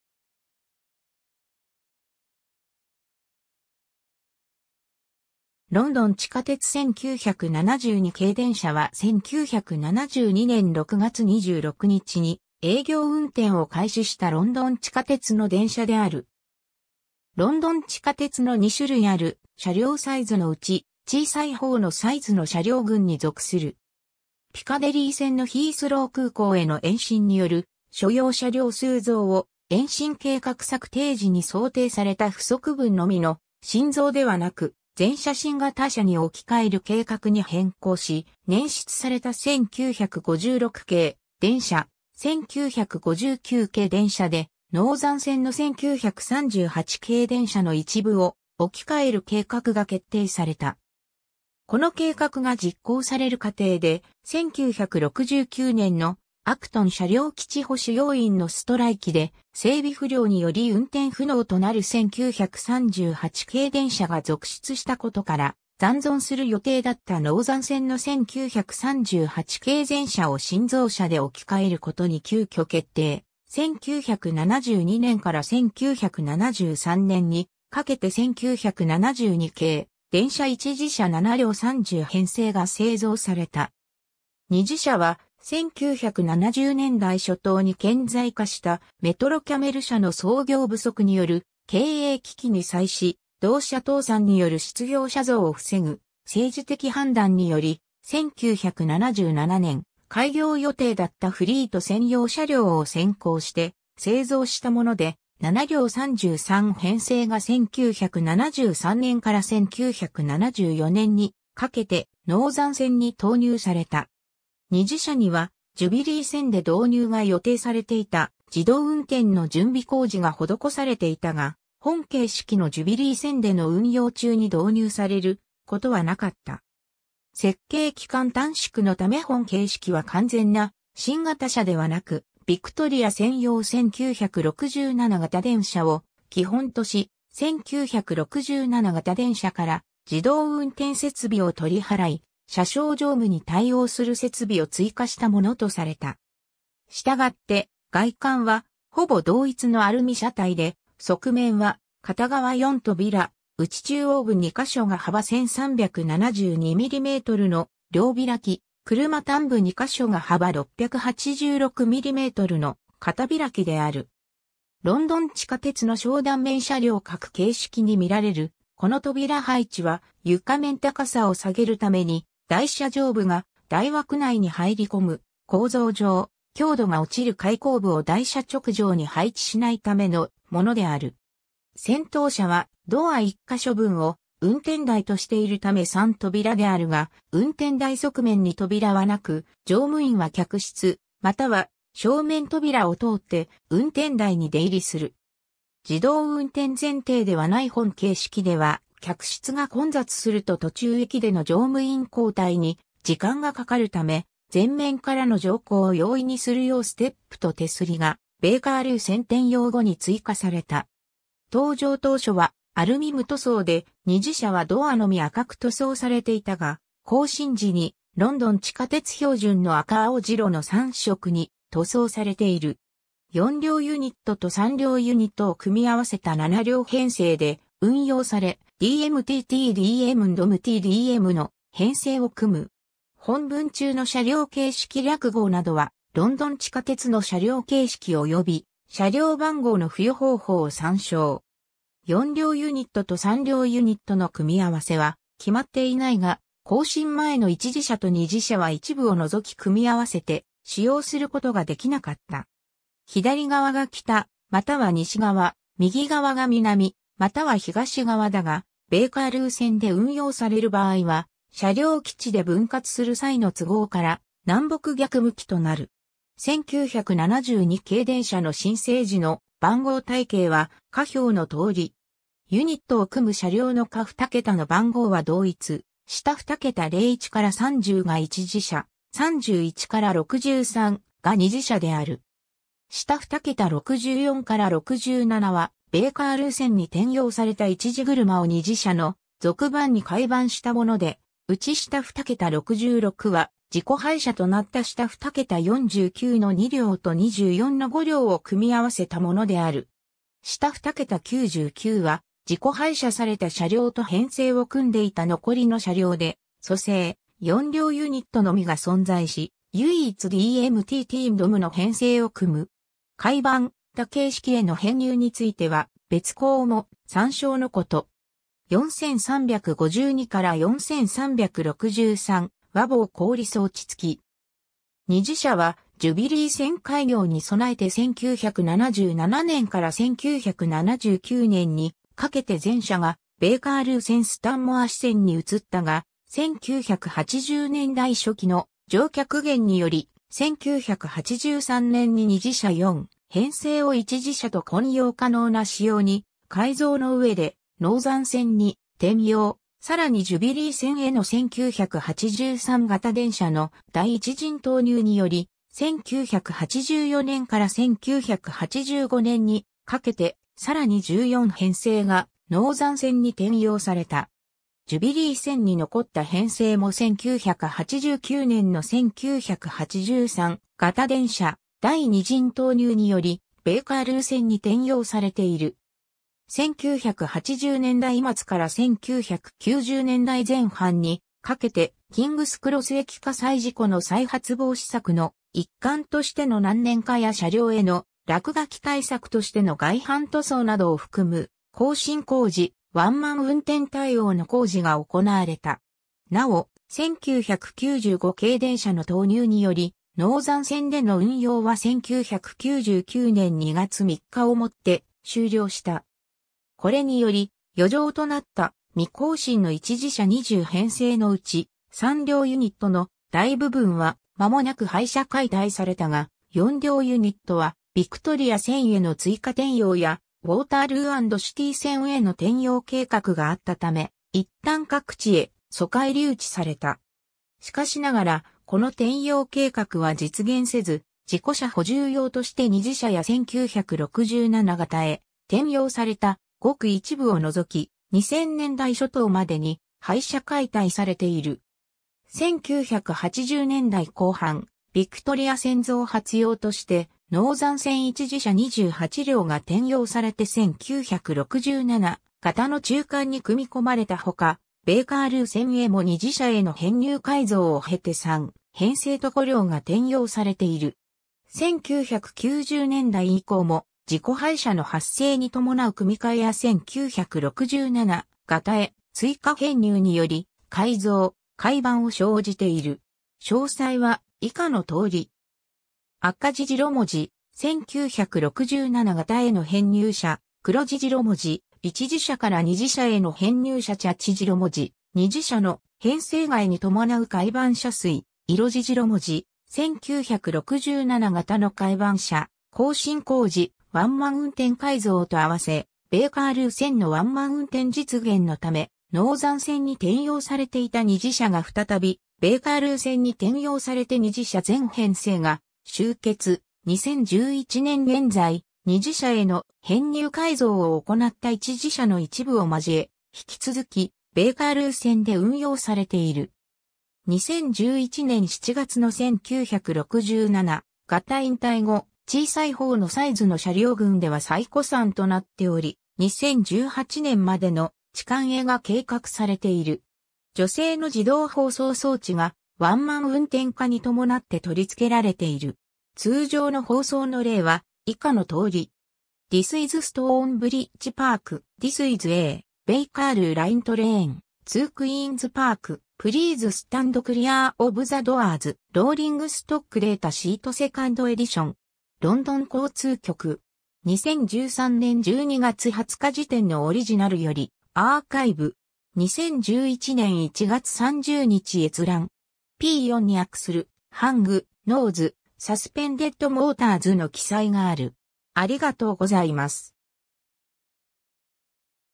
ロンドン地下鉄1972形電車
ロンドン地下鉄1972形電車(英:London Underground 1972 Stock)は1972年6月26日に営業運転を開始したロンドン地下鉄の電車である。
ロンドン地下鉄の2種類ある車両サイズのうち、小さいほうのサイズの車両群(チューブ)に属する。
 ピカデリー線のヒースロー空港への延伸による所要車両数増を、延伸計画策定時に想定された不足分のみの新造ではなく、全車新型車（1973形電車）に置き換える計画に変更し、ねん出された1956形電車・1959形電車でノーザン線の1938形電車の一部を置き換える計画が決定された。
 この計画が実行される過程で、1969年のアクトン車両基地保守要員のストライキで整備不良により運転不能となる1938形電車が続出したことから、残存する予定だったノーザン線の1938形全車を新造車で置き換えることに急きょ決定、1972年から1973年にかけて1972形電車1次車（Mk I）7両30編成が製造された。
 2次車（Mk II）は、1970年代初頭に顕在化したメトロキャメル社の操業不足による経営危機に際し、同社倒産による失業者増を防ぐ政治的判断により、1977年開業予定だったフリート線（開業前にジュビリー線に名称変更）用車両を先行して製造したもので、7両33編成が1973年から1974年にかけてノーザン線に投入された。
2次車にはジュビリー線で導入が予定されていた自動運転の準備工事が施されていたが、本形式のジュビリー線での運用中に導入されることはなかった。
 設計期間短縮のため本形式は完全な新型車ではなく、ヴィクトリア線用1967形電車を基本とし、1967形電車から自動運転設備を取り払い、車掌乗務に対応する設備を追加したものとされた。
従って、外観はほぼ同一のアルミ車体で、側面は片側4扉、うち中央部2箇所が幅1372mmの両開き、車端部2箇所が幅686mmの片開きである。
 ロンドン地下鉄の小断面車両各形式に見られるこの扉配置は、床面高さを下げるために台車上部が台枠内に入り込む構造上、強度が落ちる開口部を台車直上に配置しないためのものである。
 先頭車はドア1箇所分を運転台としているため3扉であるが、運転台側面に扉は無く、乗務員は客室または正面扉を通って運転台に出入りする。
自動運転前提ではない本形式では客室が混雑すると途中駅での乗務員交代に時間がかかるため、前面からの乗降を容易にするようステップと手すりがベーカールー線転用後に追加された。
 登場当初はアルミ無塗装で、2次車はドアのみ赤く塗装されていたが、更新時にロンドン地下鉄標準の赤青白の三色に塗装されている。
 4両ユニットと3両ユニットを組み合わせた7両編成で運用され、DM-T-T-DM+UNDM-T-DMの編成を組む。
本文中の車両形式略号などはロンドン地下鉄の車両形式および車両番号の付与方法を参照。
 4両ユニットと3両ユニットの組み合わせは決まっていないが、更新前の1次車と2次車は一部を除き組み合わせて使用することができなかった。
 左側が北または西側(Aエンド)、右側が南または東側(Dエンド)だが、ベーカールー線で運用される場合は、車両基地で分割する際の都合から南北逆向きとなる。
 1972形電車の新製時の番号体系は下表の通り。
 ユニットを組む車両の下2桁の番号は同一、下二桁01-30が1次車、31-63が2次車である。
 下二桁64-67はベーカールー線に転用された1次車を2次車の続番に改番したもので、うち下二桁66は事故廃車となった下二桁49の2両と24の5両を組み合わせたものである。
下二桁99は事故廃車された車両と編成を組んでいた残りの車両で組成、4両ユニットのみが存在し、唯一DM-T-T-UNDMの編成を組む。
改番・他形式への編入については別項も参照のこと。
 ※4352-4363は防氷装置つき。
 2次車はジュビリー線開業に備えて1977年から1979年にかけて全車がベーカールー線スタンモア支線（後のジュビリー線の北半分）に移ったが、1980年代初期の乗客減により、1983年に2次車4編成を1次車と混用可能な仕様に改造の上でノーザン線に転用、さらにジュビリー線への1983形電車の第1陣投入により、1984年から1985年にかけてさらに14編成がノーザン線に転用された。
ジュビリー線に残った編成も1989年の1983形電車第2陣投入によりベーカールー線に転用されて...